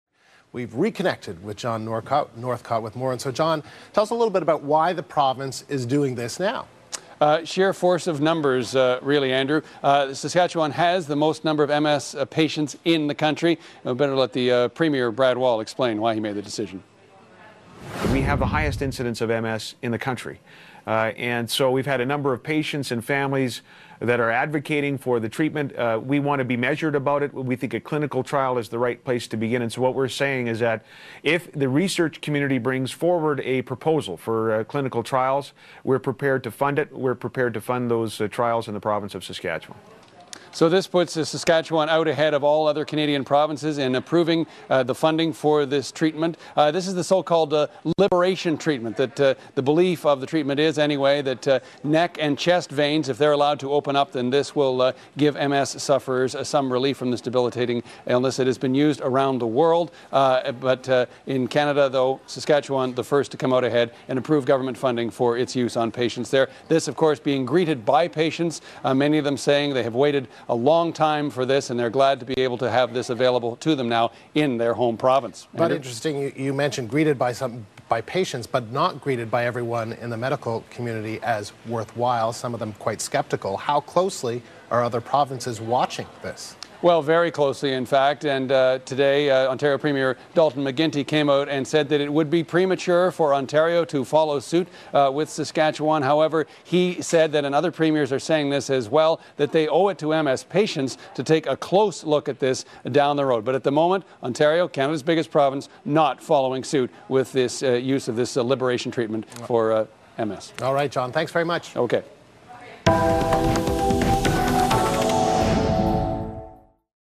CBC Multiple Sclerosis story
A clip from my interview with the CBC about funding for research of Liberation Treatment therapy.